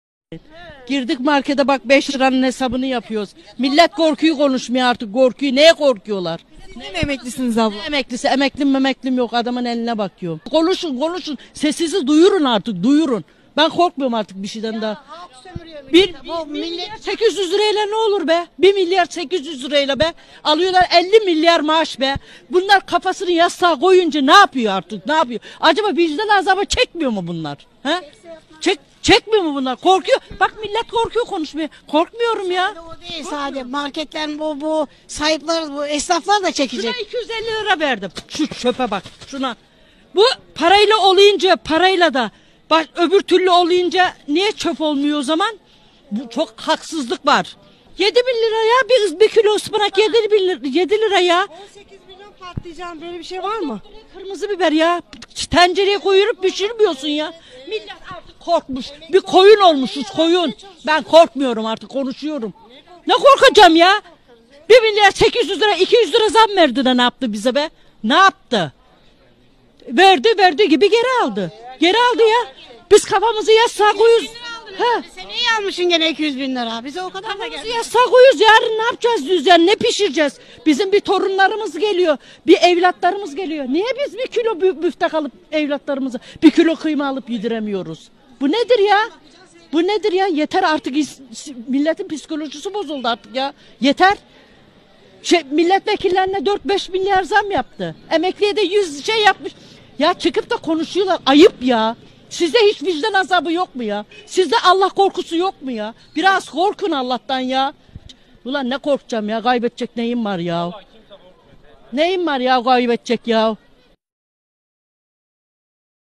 İş İşten Geçtikten Sonra Uyanan AKP'li Koyunlar
Mucize gerçekleşti!

Koyunlar konuşmaya başladı. Dünyanın gözü Türkiyede.. Koyunların aç kalınca konuşma denemeleri yapması bilim adamlarını şaşırttı.